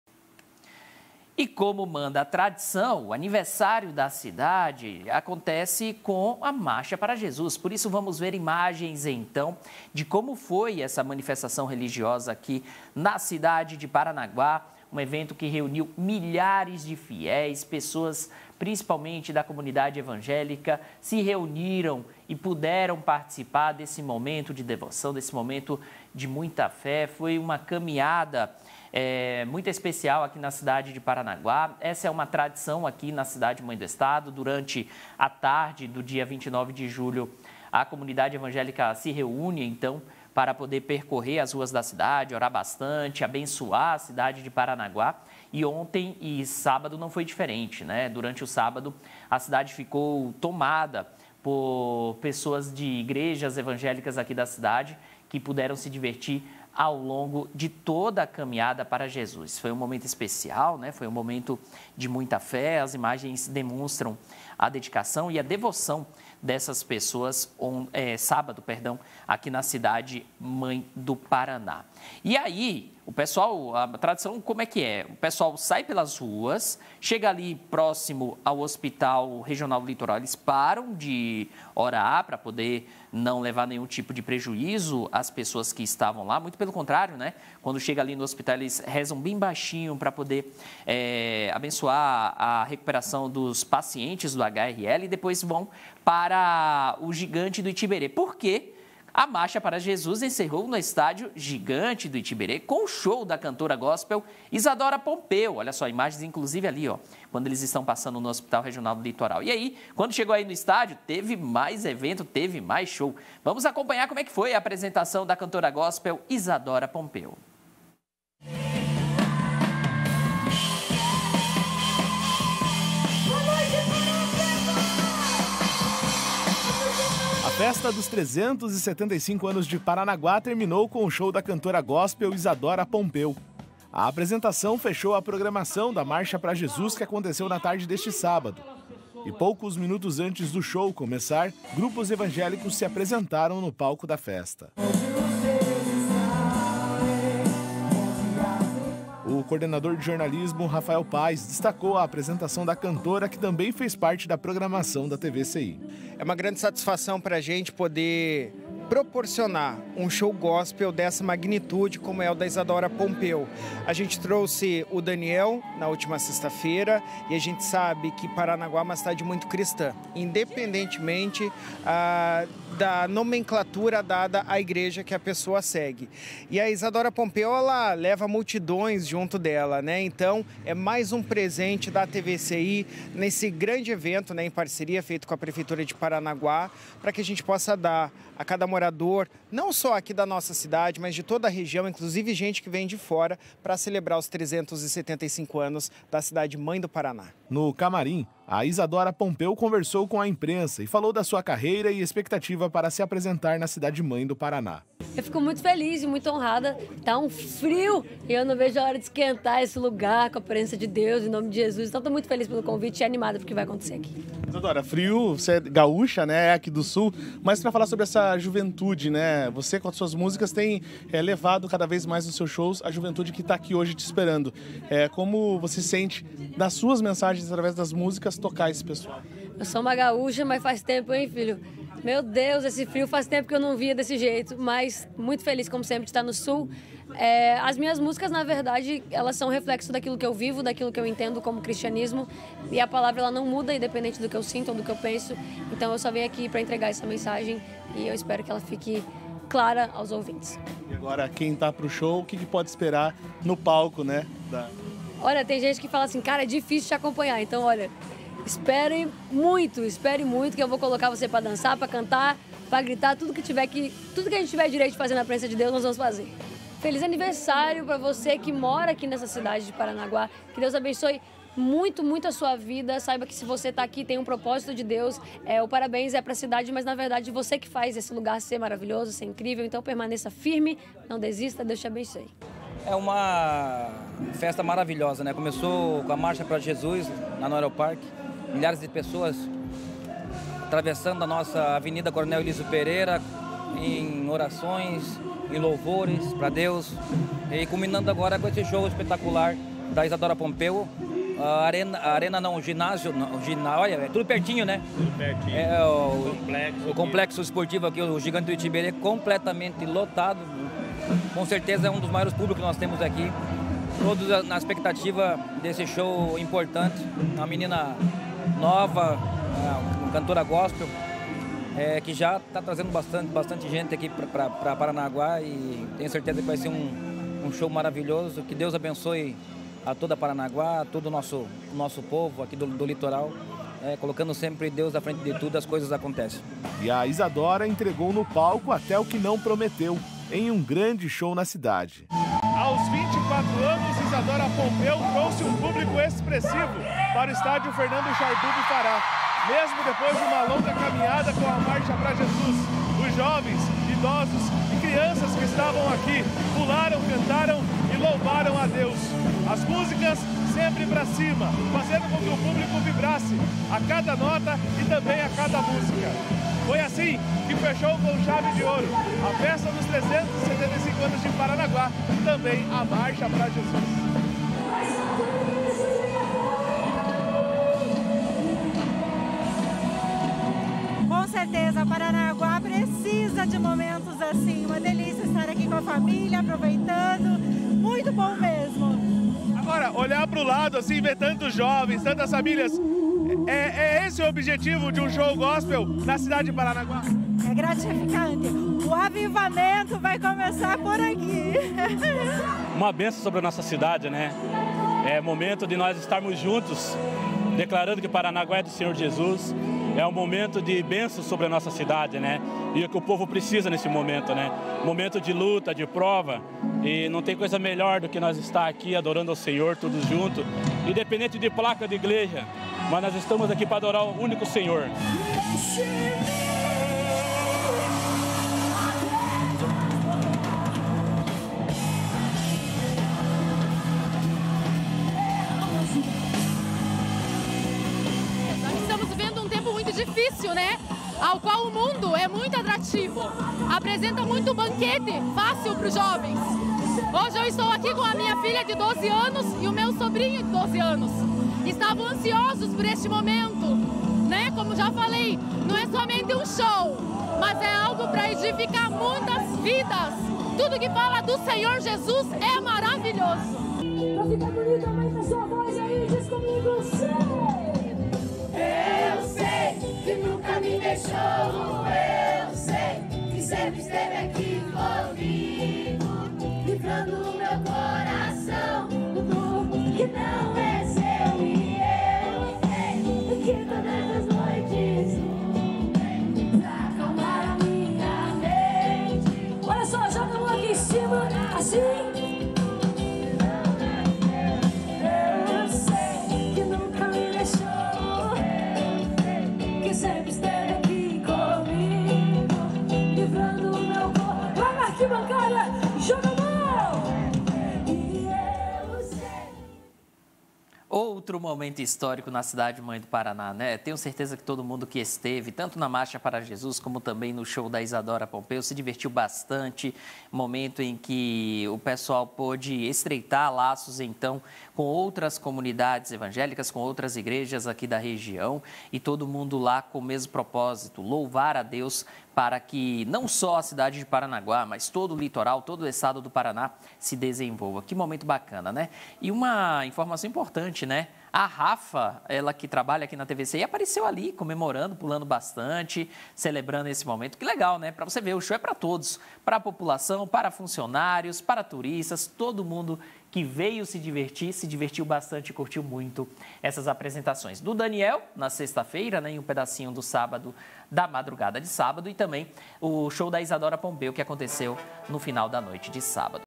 375 ANOS DE PARANAGUÁ
Marcha para Jesus encerrou com apresentação da cantora gospel Isadora Pompeo